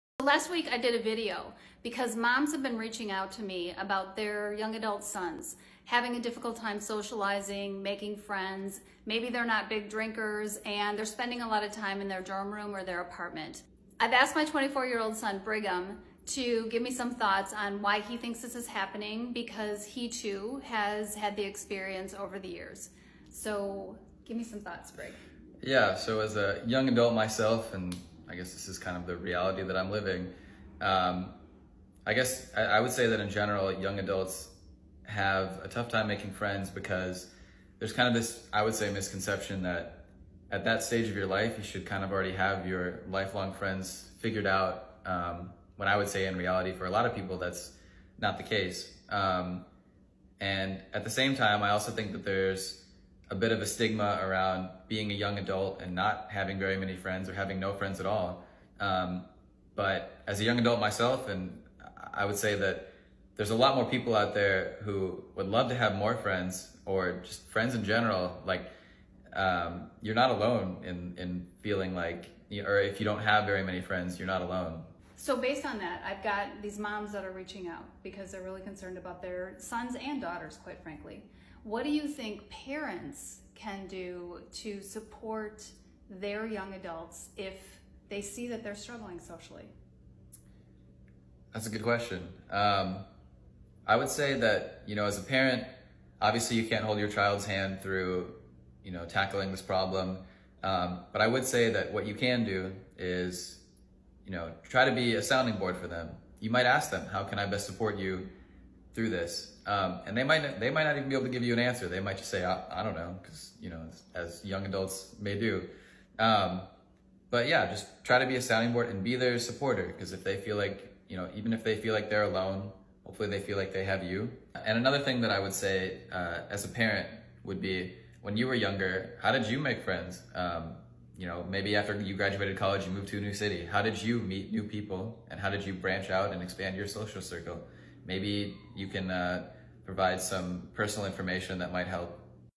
Mom, if you are worried about your young adults lack of social life, you arent alone. I see this...
I am a certified parenting coach and the founder of the Real Life Parent Guide, which offers one-on-one coaching.